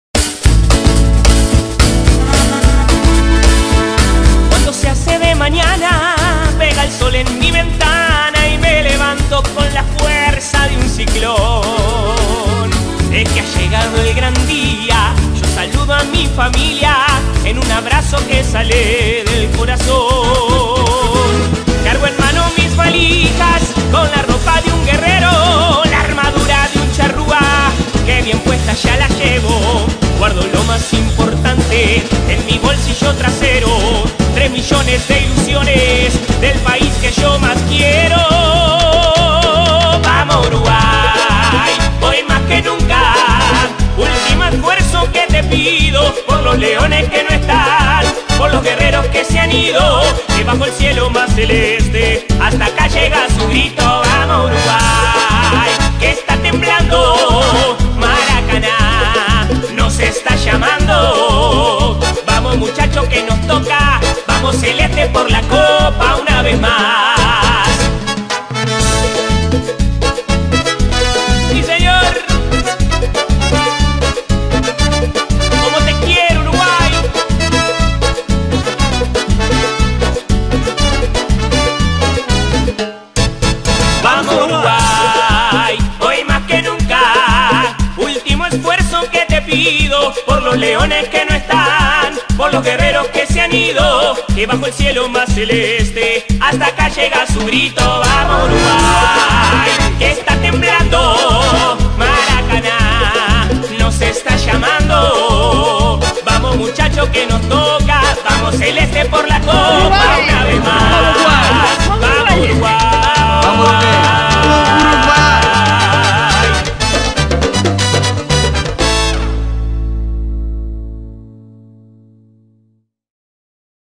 ¡Vamo' Uruguay!
El grito que suena desde Rusia y se hace eco en todas partes del Mundo donde haya un hijo o hija de Obdulio presente: ¡vamo' Uruguay!